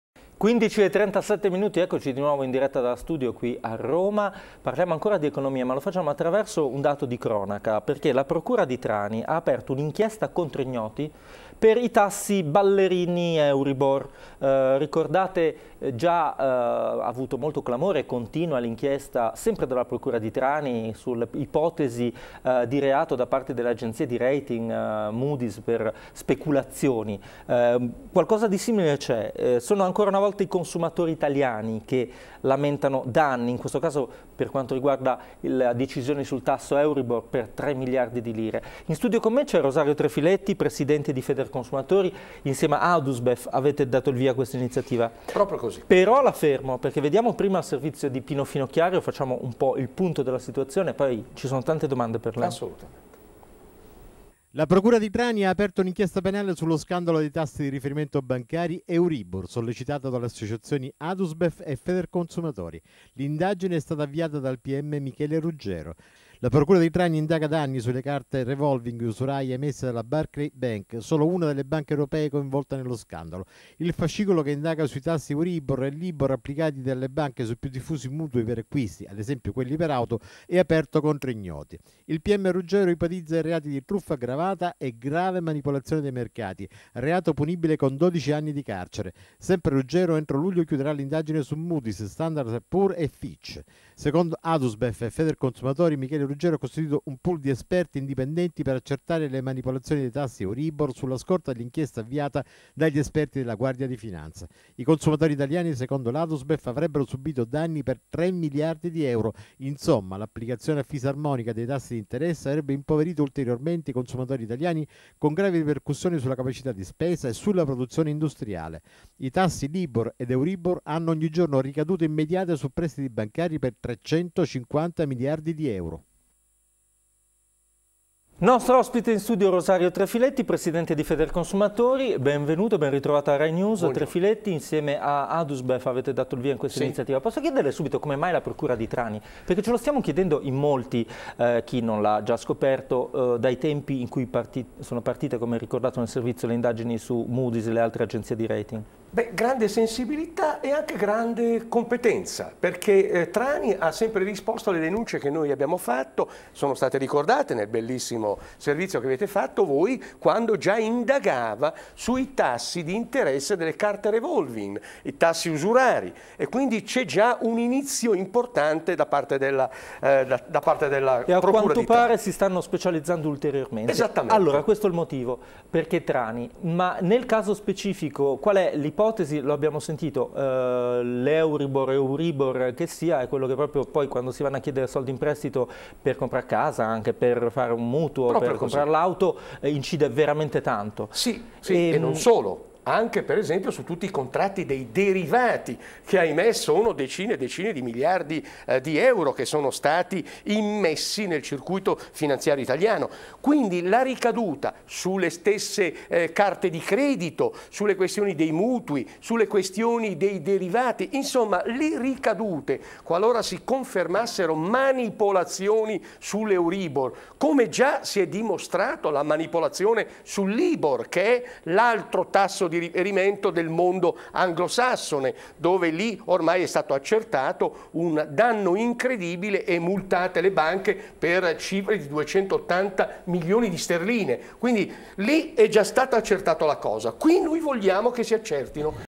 euribor trefiletti 200712
Rosario Trefiletti intervistato da Alessandro Baracchini commenta le indagini sui tassi Euribor dopo un servizio di Pino FInocchiaro sull'inchiesta della procura di Trani. Un'esclusiva di Rai News.